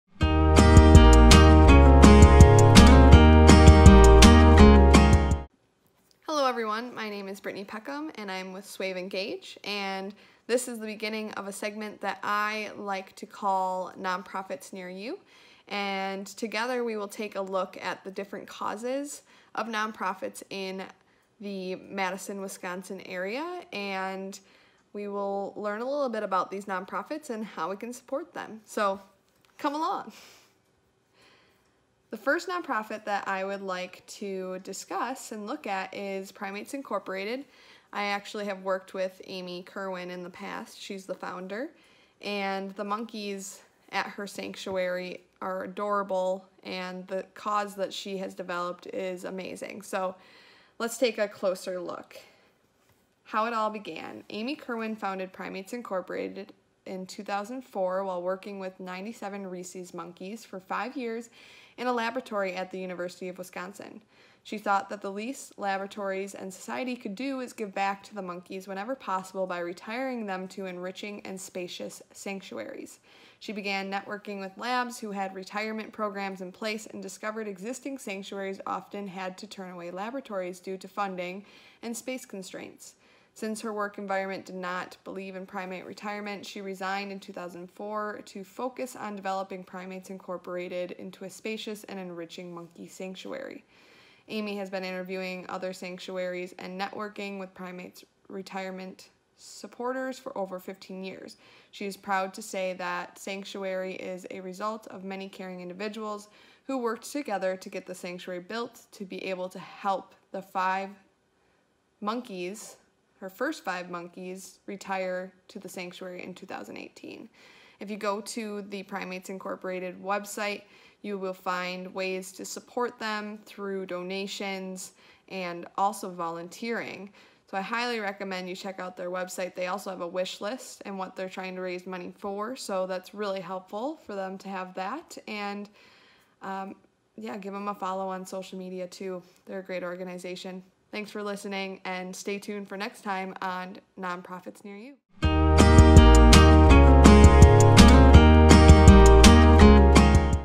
Nonprofits Near You: Primates Incorporated
This is the beginning of fun but informative segment called “Nonprofits Near You”!

In this series, we will highlight nonprofits in different parts of the country, what they do, and how you can support them.

This week, we are taking a journey to Westfield, WI., where monkeys are saved and cared for in a safe space, the Primates Incorporated sanctuary.

Amy Kerwin founded Primates Incorporated in 2004 while working with 97 rhesus monkeys for five years in a laboratory at the University of Wisconsin. Amy felt the monkey’s deserved a life after research.

She discovered other monkey sanctuaries and set out to join this movement.